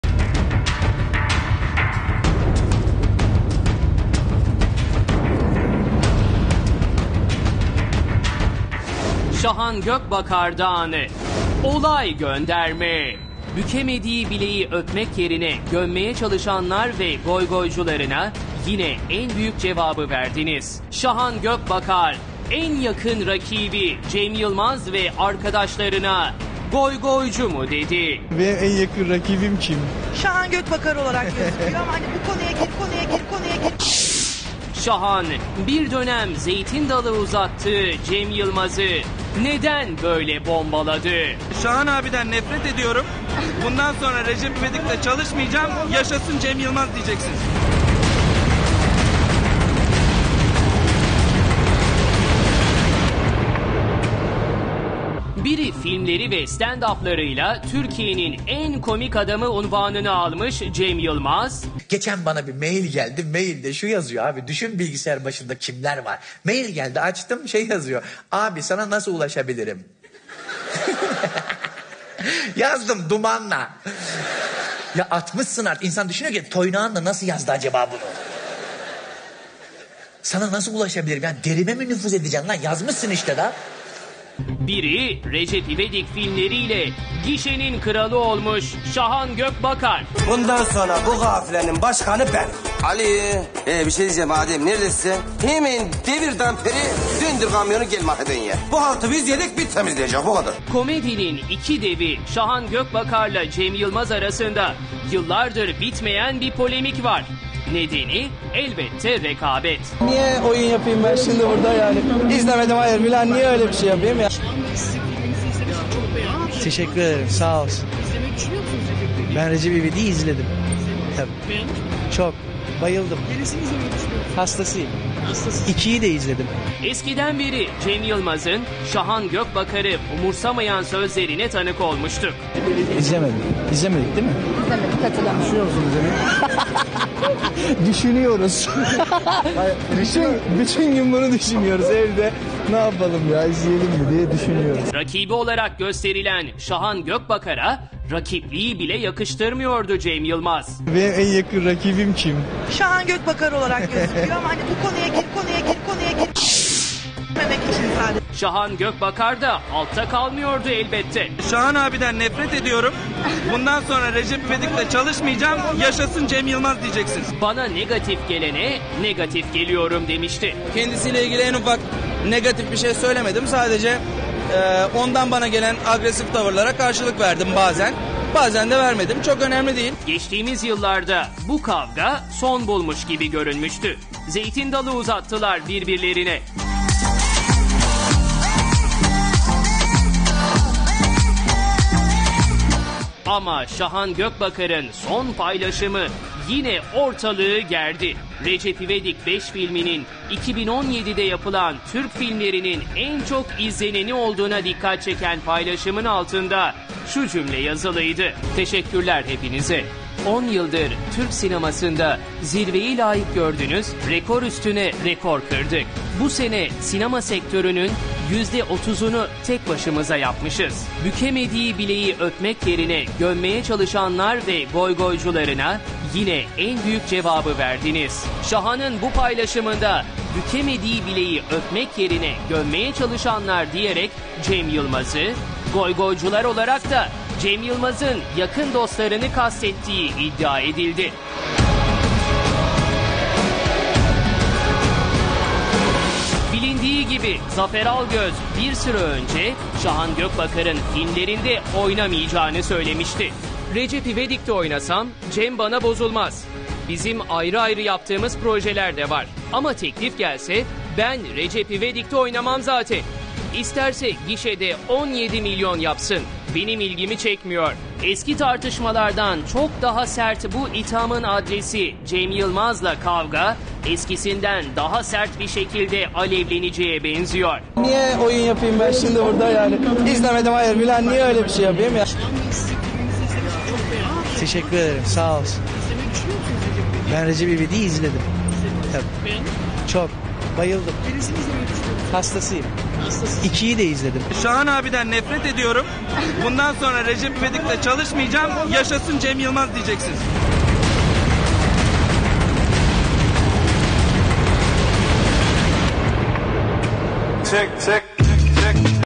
Şahan Gökbakar'dan olay gönderme!
Şahan Gökbakar'ın son paylaşımı Cem Yılmaz ile "goygoy" kavgasına sebep oldu.